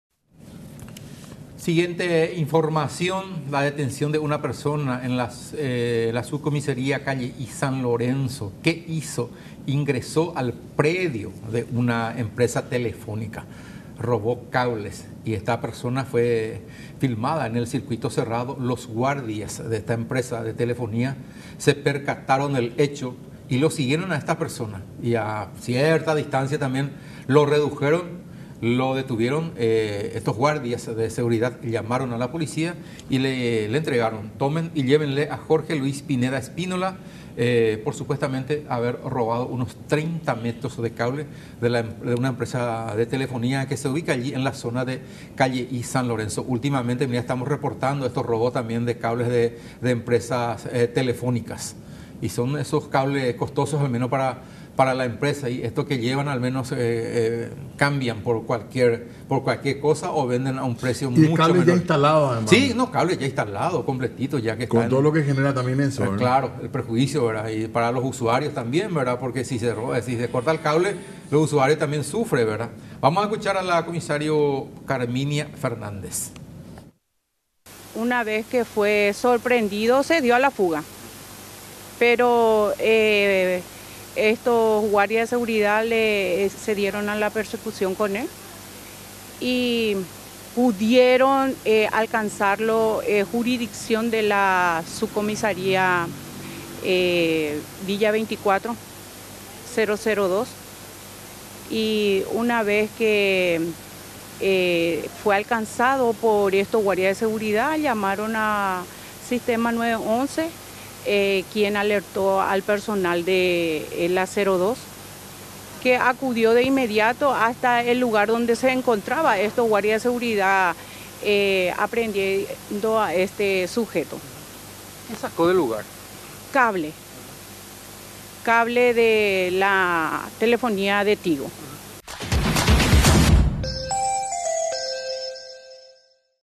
Roba cable de empresa telefónica
Un sujeto de 34 años identificado como Jorge Luis Pineda Espínola fue detenido por supuestamente haberse llevado 30 metros de cable de la antena de una empresa de telefonía. Los guardias privados lo habían visto en circuito cerrado y salieron en busca del presunto ladrón, que fue seguido y reducido en otro sitio fuera del local de la empresa. El hecho ocurrió en en el barrio Laurelty de San Lorenzo.